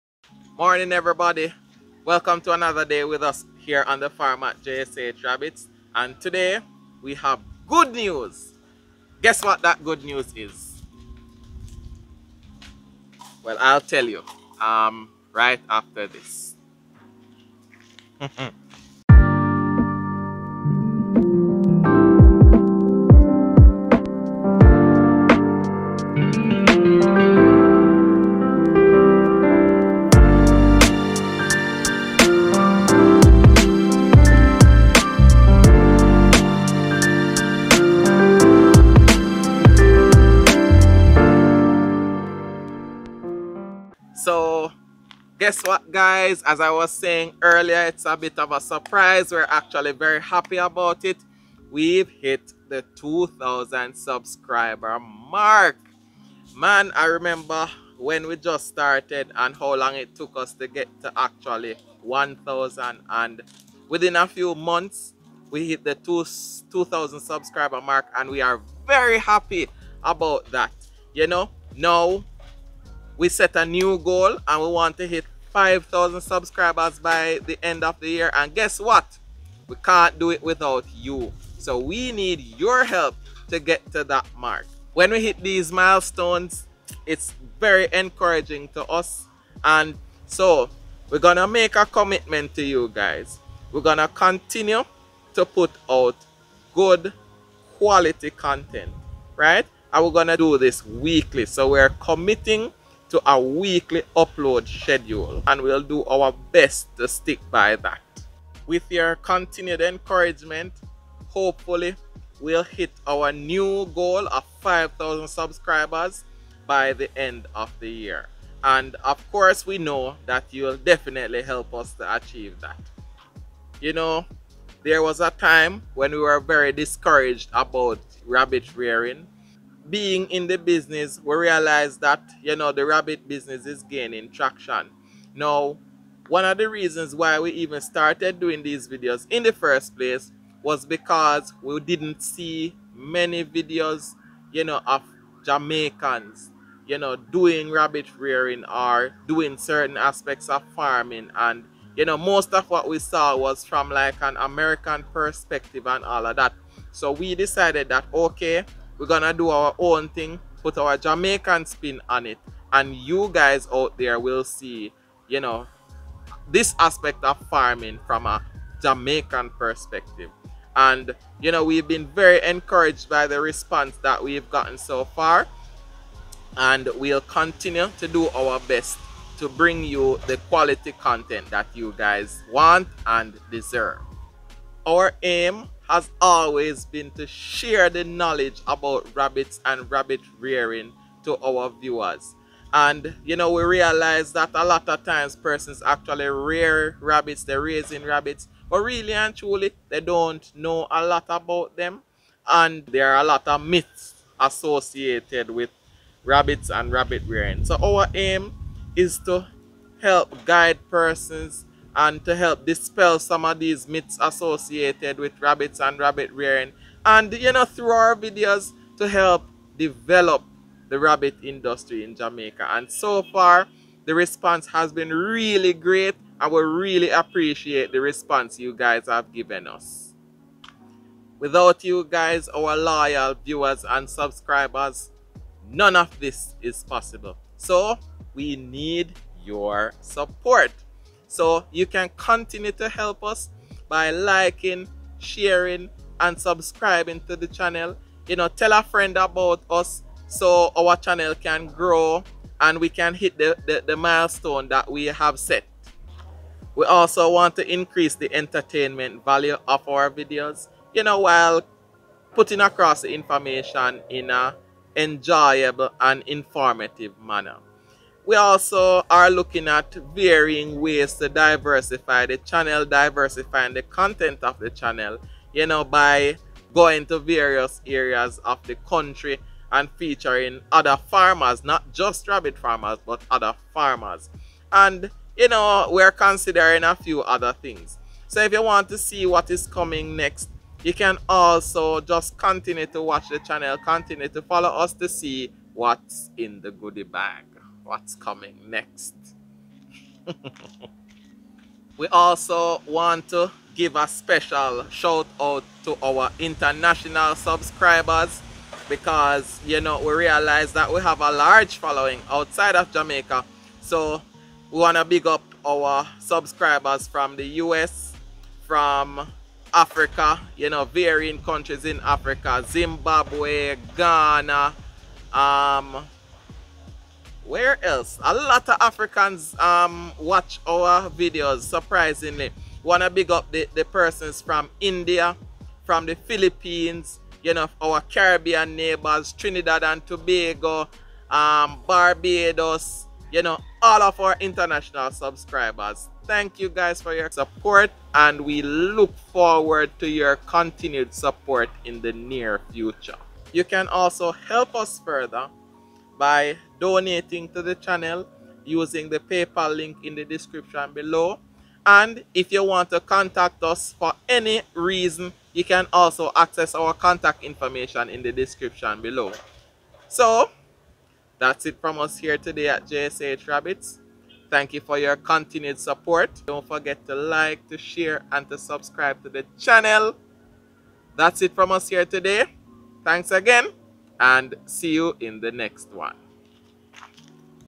Jamaica Rabbit Farming: New Subscriber Mark!!!
This is just a thank you video to you our loyal viewers and subscribers!! Thank you for sticking with us throughout this journey!! We've had a lot of ups and downs in rabbit rearing but still we have managed to build the business to a level that we can be proud of. We still have a long way to go and we hope to have you along for the ride.

It would help to push us a lot further!! Any help would be appreciated.